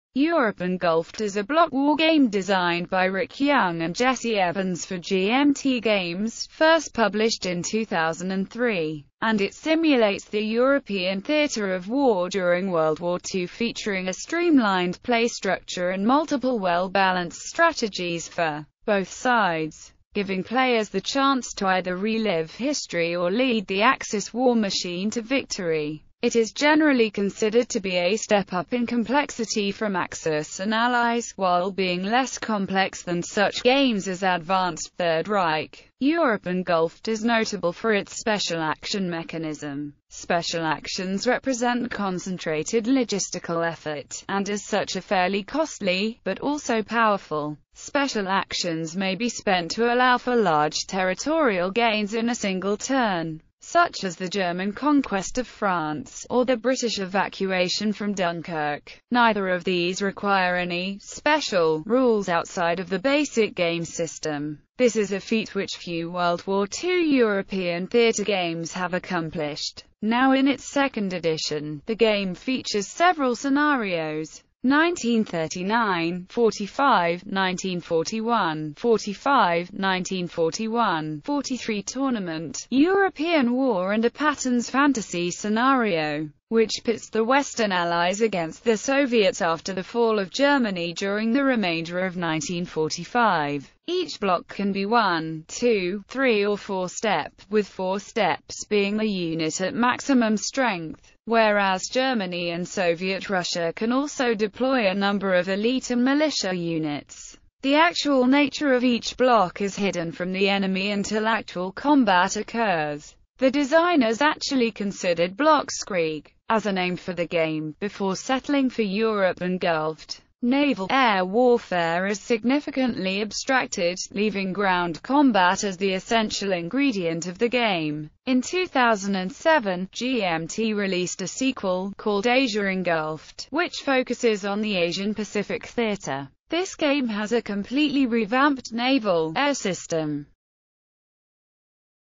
Europe Engulfed
Europe Engulfed is a block wargame designed by Rick Young and Jesse Evans for GMT Games, first published in 2003, and it simulates the European theater of war during World War II featuring a streamlined play structure and multiple well-balanced strategies for both sides, giving players the chance to either relive history or lead the Axis war machine to victory.It is generally considered to be a step up in complexity from Axis & Allies, while being less complex than such games as Advanced Third Reich.